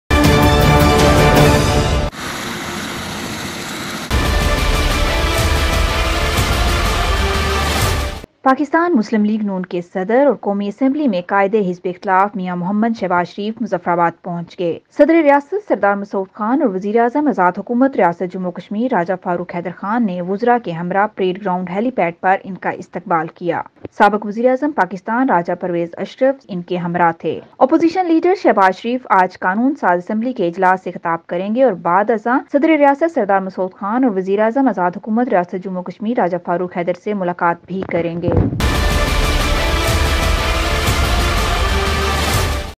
President PML N Mian Shahbaz Sharif on his arrival in Muzaffarabad welcomed and met with delegations
PML N#Mian Shahbaz Sharif# Muzaffarabad azad kashmir#